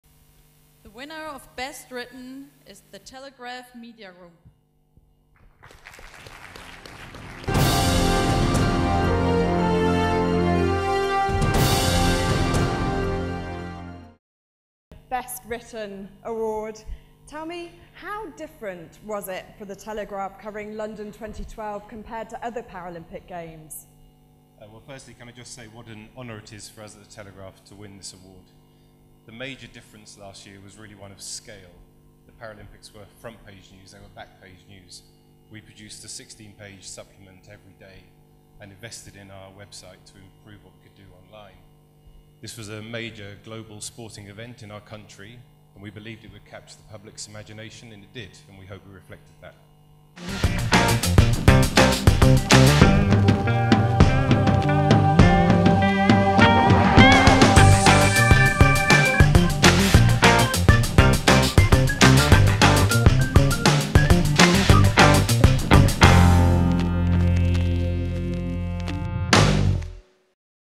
Telegraph Media Group wins Best Written Paralympic Award
Four organisations received Paralympic Media Awards for showcasing extraordinary coverage of the London 2012 Paralympic Games -- three of these award winners coming from Great Britain and the other from Canada.

Channel 4 won the award for Best Broadcast after showcasing more than 500 hours of London 2012 coverage and transforming perceptions with their advertising campaigns for the Games.

Telegraph Media Group won the Best Written (print and online) award, BBC World Service won Best Radio and Edmonton Journal photographer Larry Wong won for Best Photo for his stunning image of Canadian swimmer Benoit Huot celebrating victory.